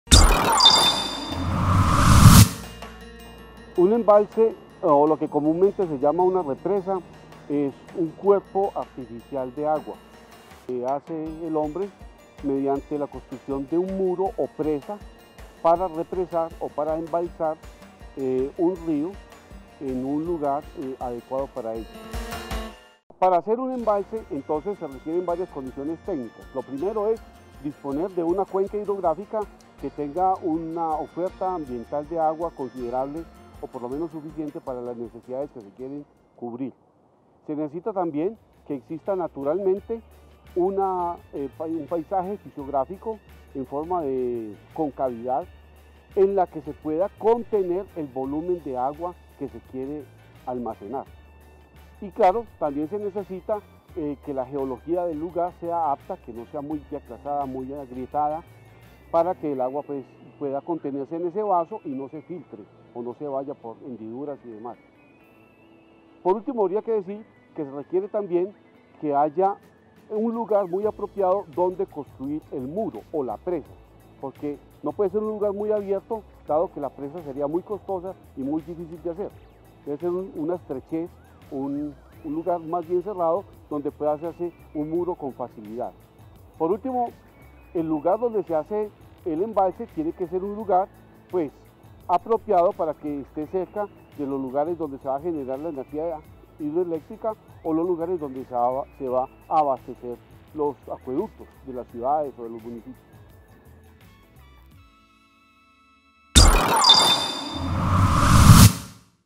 Condiciones geológicas para la construcción de una represa
Para realizar la construcción de una represa, son necesarias varias condiciones del terreno que permitan el adecuado funcionamiento.